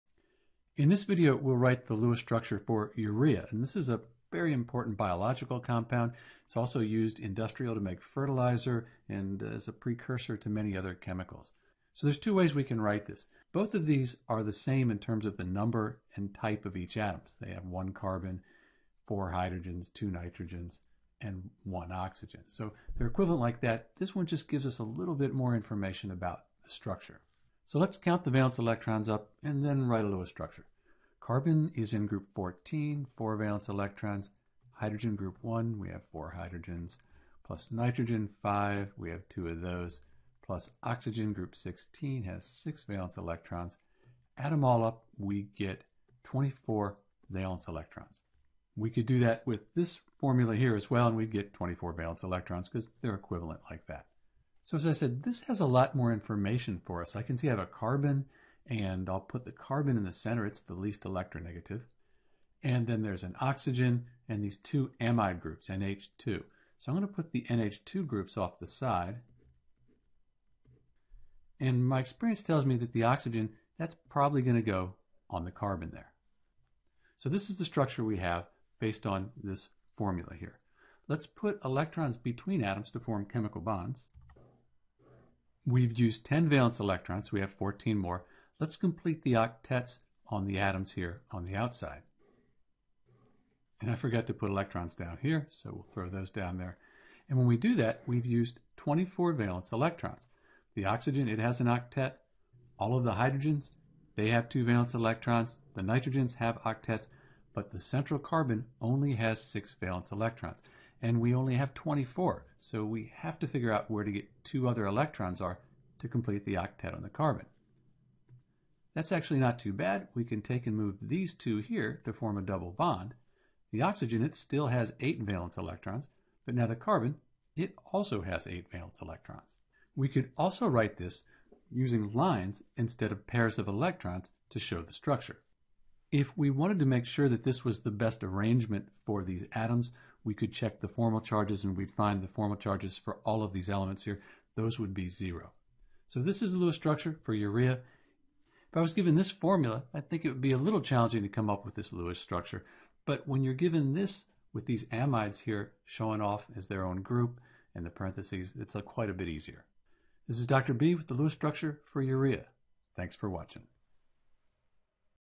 How to Draw the Lewis Dot Structure for CH4N2O / CO(NH2)2 : Urea
A step-by-step explanation of how to draw the CH4N2O Lewis Dot Structure (Urea).

For the CH4N2O structure use the periodic table to find the total number of valence electrons for the CH4N2O molecule. Once we know how many valence electrons there are in CH4N2O we can distribute them around the central atom with the goal of filling the outer shells of each atom.

In the Lewis structure of CH4N2O structure there are a total of 24 valence electrons. CH4N2O is also called Urea.